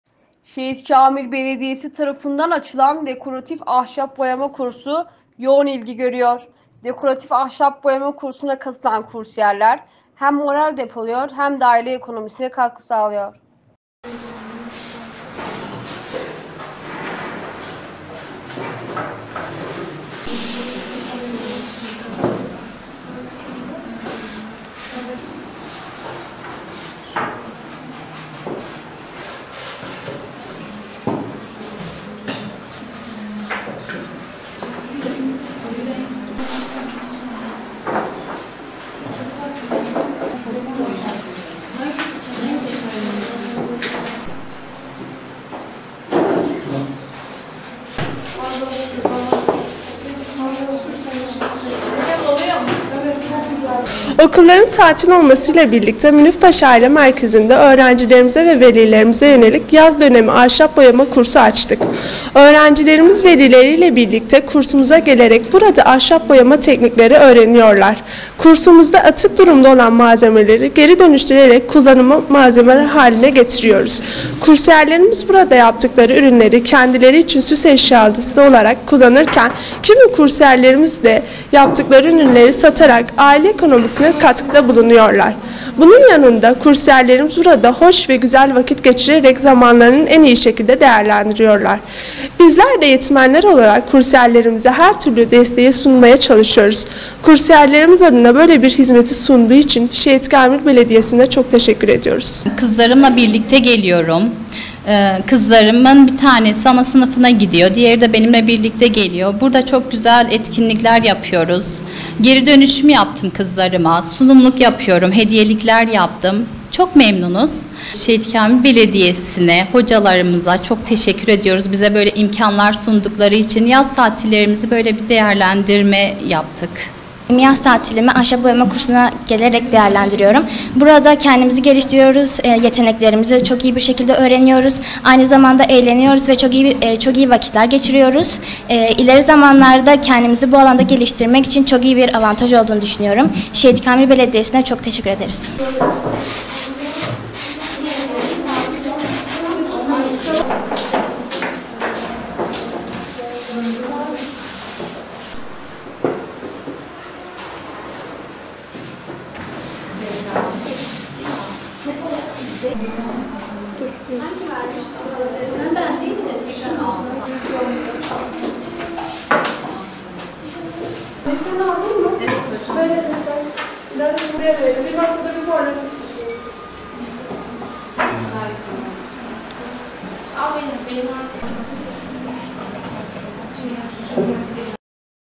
Dekoratif Ahşap Boyama Kursu Yoğun İlgi Görüyor
Şehitkamil Belediyesi tarafından açılan Dekoratif Ahşap Boyama Kursu, yoğun ilgi görüyor.